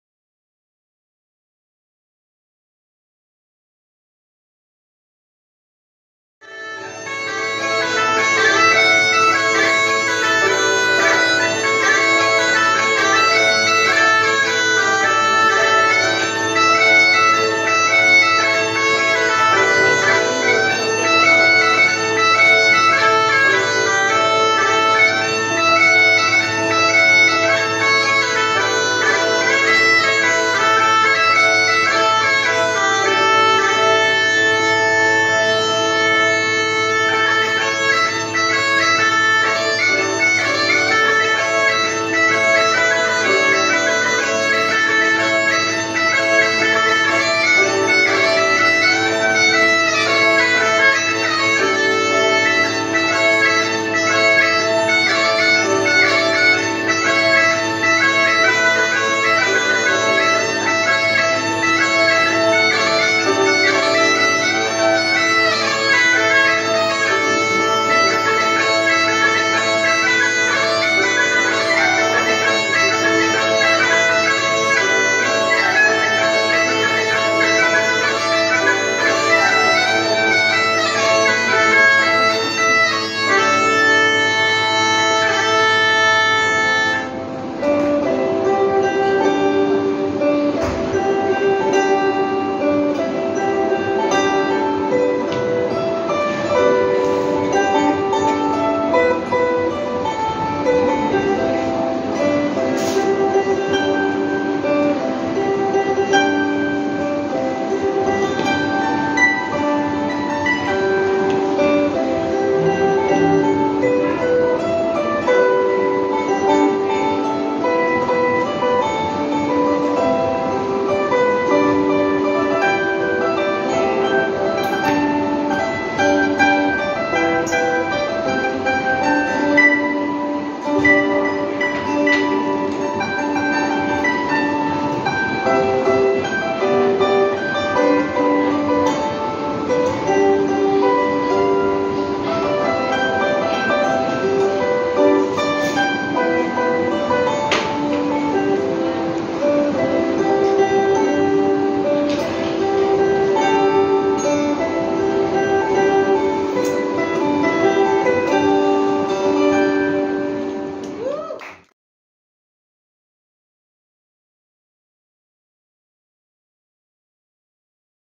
Break time music service (09/Oct/2023)
Pipe & Clarsach music service for the break time at Scottish Dance Session (09/Oct/2023) by Bangkok St Andrews Society

Clif /Scottish small pipe & Tomo /Clarsach